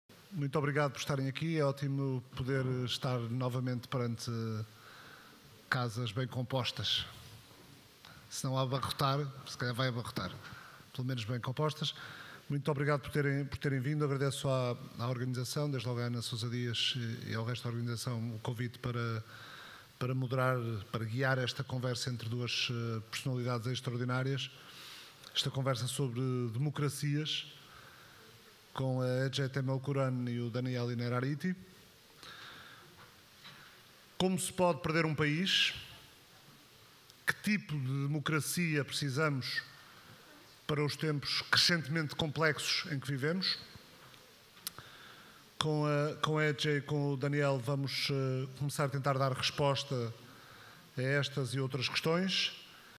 "Fim das Democracias" \\\ Mesa 13, FOLIO FESTIVAL 2021
FOLIO AUTORES 2021 - Mesa 13, "Fim das Democracias",
Ece Temelkuran conversa com Daniel Innenarity.
Moderação de Ricardo Alexandre.